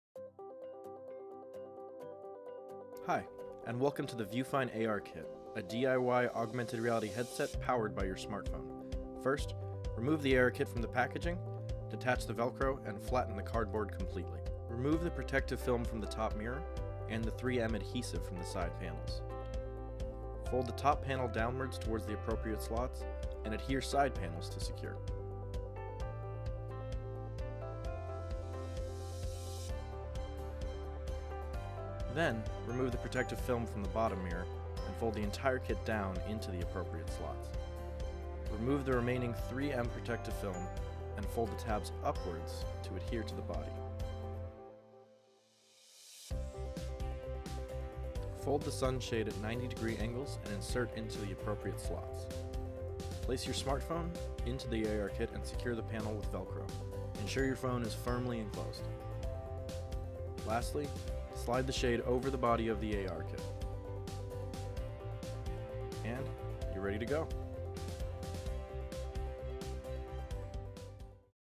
Vufine AR Kit Assembly Instructions
The Vufine AR Kit is an easily assembled DIY headset that is powered by your smartphone. Simply select the desired AR application and strap your phone into the AR Kit to experience it as a full-blown augmented reality headset. The optics present your screen in a crisp 75 degree field of view. By avoiding the limitations of stereoscopic optics, the Vufine AR Kit utilizes the full resolution of your smartphone and is completely open source for AR apps. Whether to use with existing apps or to encourage development of your own, the Vufine AR Kit is able to enhance your augmented reality experience.

The Vufine AR Kit is currently available through our online store for $14.99.